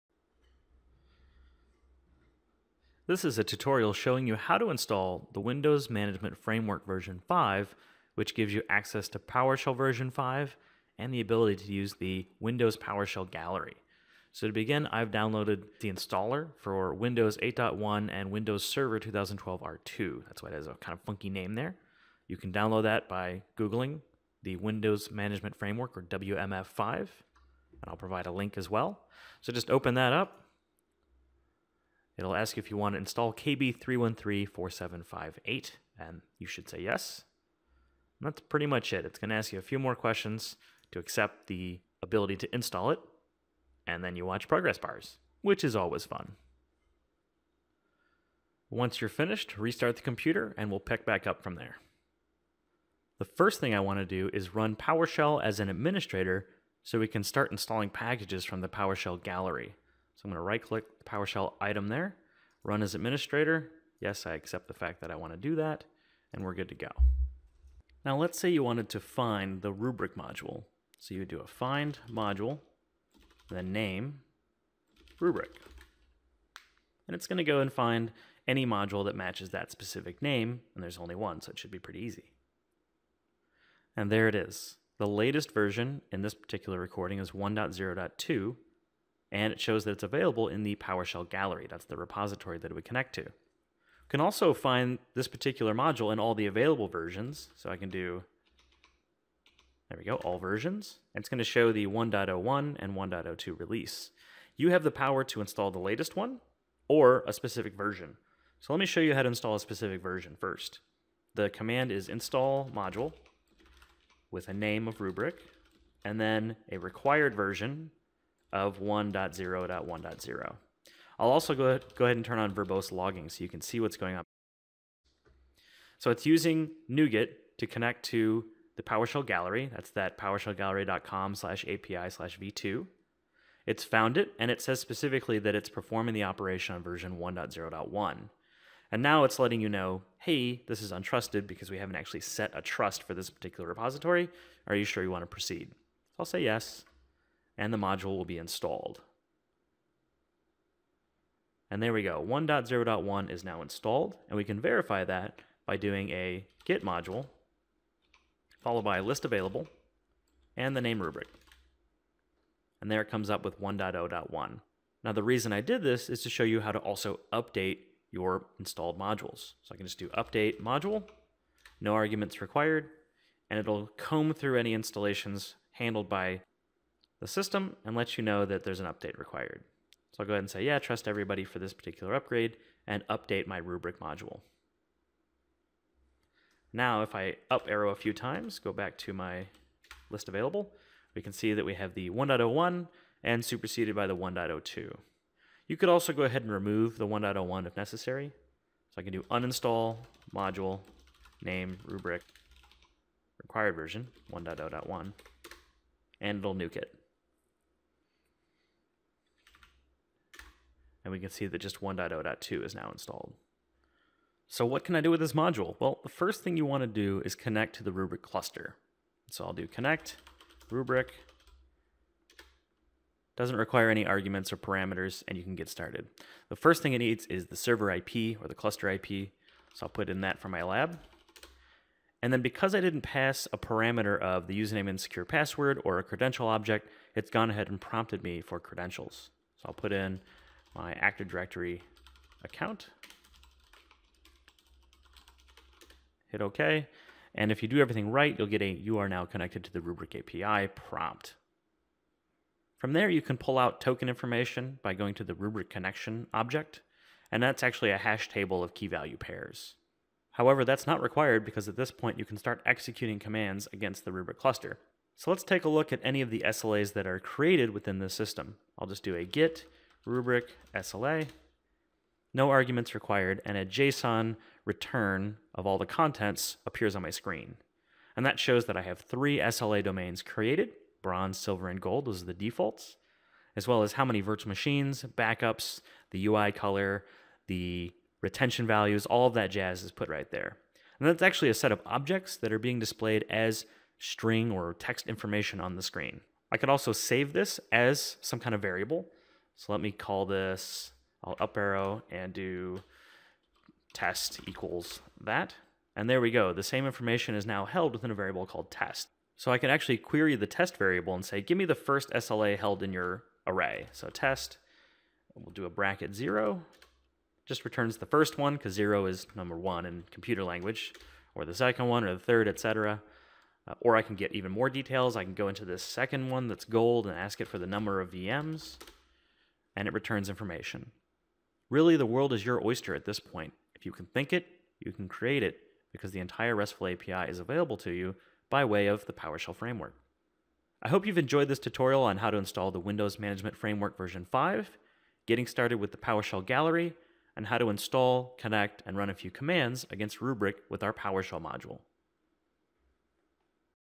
An Introduction to WMF 5, the PowerShell Gallery, and the Rubrik Module
In this short tutorial, Chris Wahl, Technical Evangelist at Rubrik, walks through the steps required to install the Windows Management Framework 5.0, connect to the PowerShell Gallery, and begin using the Rubrik PowerShell module.